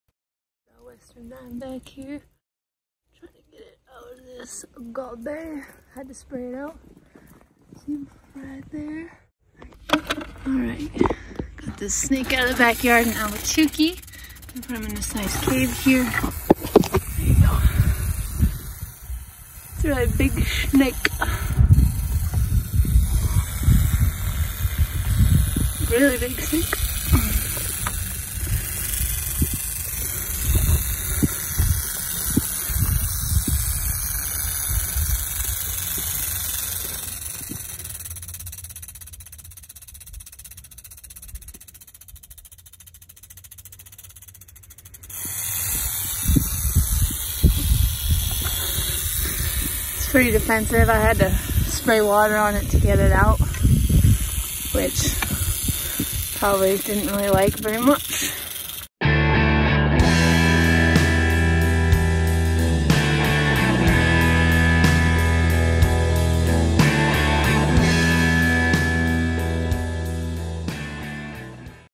Big Western Diamondback Rattlesnake in Ahwatukee
Marissa helped get this big Western Diamondback Rattlesnake out of an Ahwatukee backyard. It’s important during the hot time of year to relocate them to very specific cover. Simply moving them elsewhere and putting them in some bushes is a death scenario, and more attention to the specific microhabitat requirements is required.